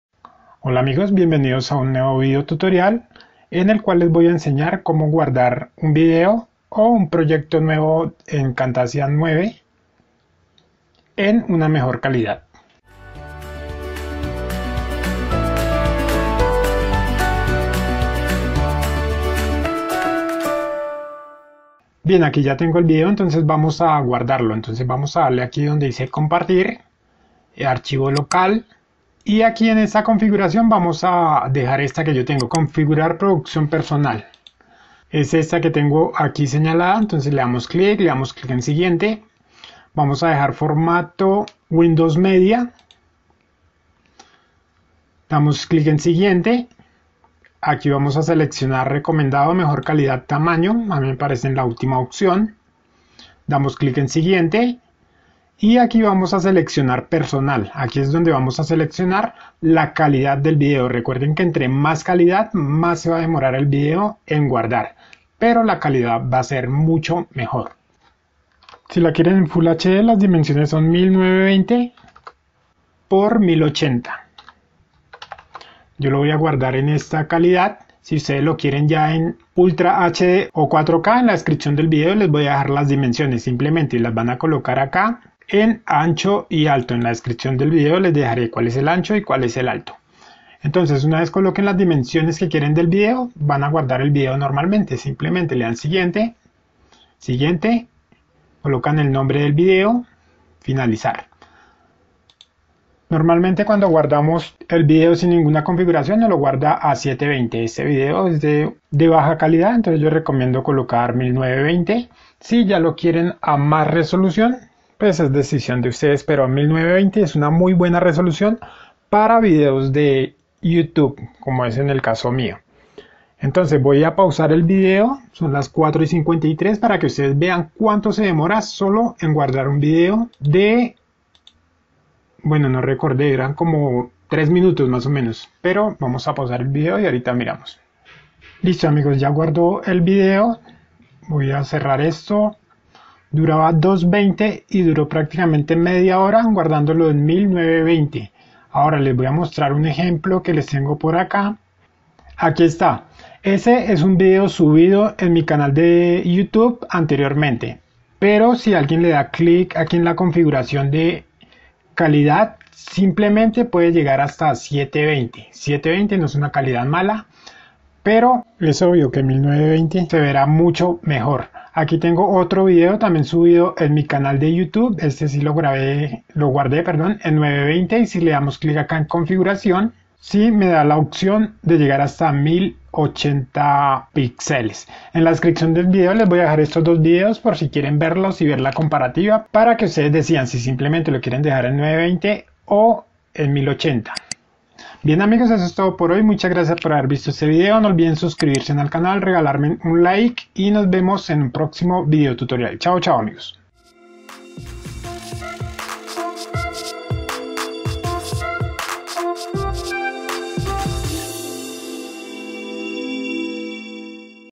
🎦 Como GUARDAR Vídeos En CAMTASIA Studio 9 en HD / Full HD / 4K ¡Renderiza tus videos ahora mismo!
¿Quieres que tus videos se vean impresionantes en cualquier pantalla? En este tutorial te enseñamos cómo guardar tus videos en HD, Full HD y 4K en Camtasia Studio 9. Aprende a optimizar el tamaño de tus videos para que se vean increíbles en cualquier dispositivo, desde tu teléfono móvil hasta tu TV de pantalla grande.

Además, te explicamos todo sobre el proceso de renderizado y cómo ajustar las configuraciones para obtener los mejores resultados. Sabemos que cada segundo cuenta, por eso te brindamos consejos para reducir el tiempo de renderizado y acelerar el proceso en equipos de gama baja.

Con nuestro tutorial, aprenderás a guardar tus videos en Camtasia Studio 9 de la manera más efectiva y eficiente, logrando una calidad excepcional en cada proyecto. ¡No pierdas más tiempo y conviértete en un experto en la creación de videos en alta definición!

Eres nuevo con camtasia?
Abre un Documento PowerPoint y revisa que no te pase esto a lo mejor no lo quieras

0:00 Intro
0:21 como guardar un video en camtasia studio 9
0:54 como seleccionar a calidad de video en camtasia studio 9
2:17 cuanto tardo en guardar un video en 1920 x 1080 en camtasia studio 9
3:13 Agradecimientos
TAMAÑO RESOLUCIÓN DE VÍDEOS EN CAMTASIA 9

2160p  x 3840 x 2160 pixeles (4K)
1440p  x 2560 x 1440 pixeles (2K)
1080p  x 1920 x 1080 pixeles (Full HD)
720p  x 1280 x 720 pixeles     (HD)
640 x 480 píxeles
640 x 360 pixeles
426 x 240 pixeles
256 x 144 pixeles